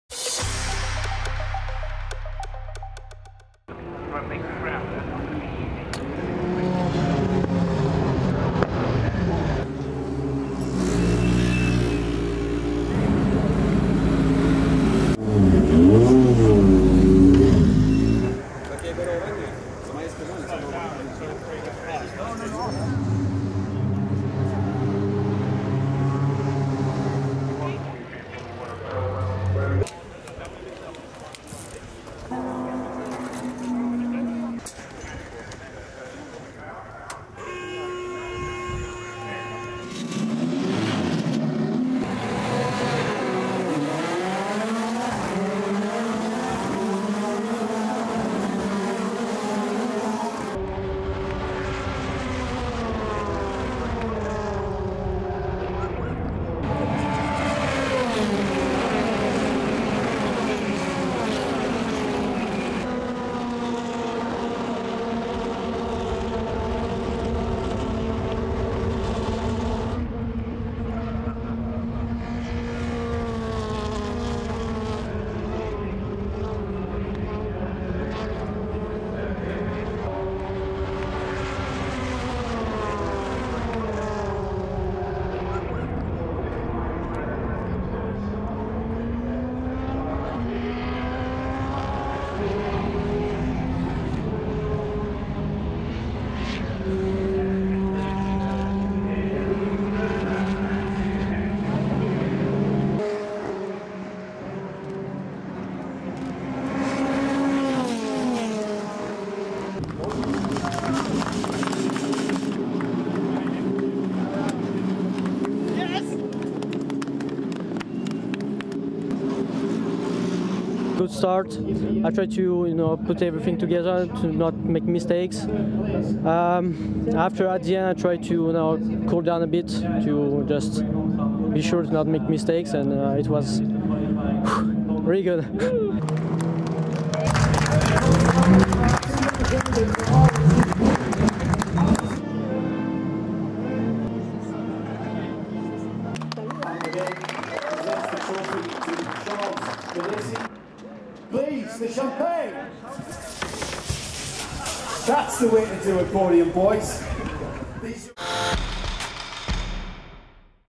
2018 Formula Renault Eurocup - Round 3 - Silverstone - Race 2 Highlights
After inheriting the pole this morning, France’s Charles Milesi (R-ace GP) controlled race two of the Formula Renault Eurocup at Silverstone to take his first win in the category. Logan Sargeant (R-ace GP) made it the third one-two finish for R-ace GP this season. Another third place result for Yifei Ye (Josef Kaufmann Racing) sees the Chinese driver increases his lead to 10 points heading to the next round in Monaco.